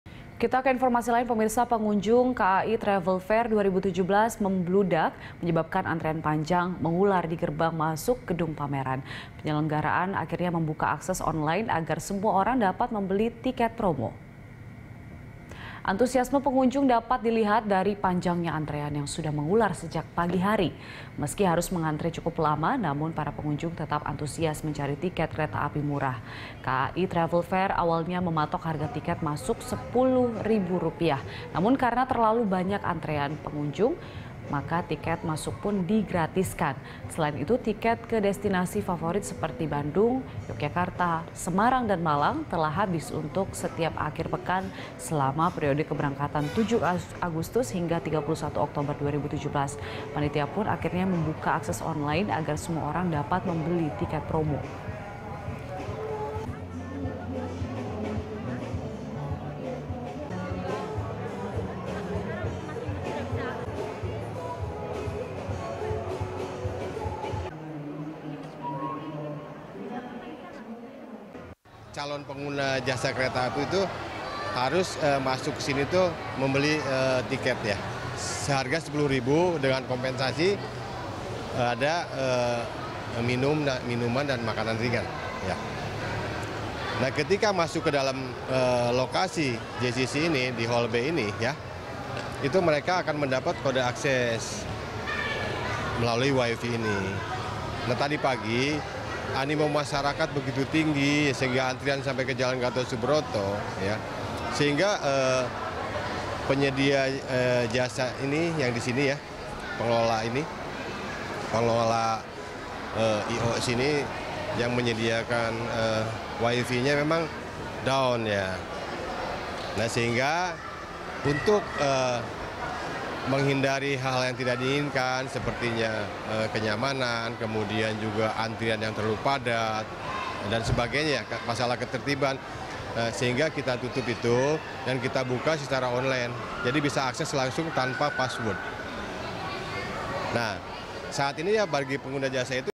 Pengunjung KAI Travel Fair 2017 Berdesakan Demi Tiket Promo
Pengunjung KAI Travel Fair (KAITF) 2017 membludak hingga menimbulkan antrean panjang mengular di gerbang masuk gedung pameran. Penyelenggara akhirnya membuka akses online agar semua orang dapat membeli tiket promo.

Facebook.com/BeritaSatuTV
Youtube.com/BeritaSatu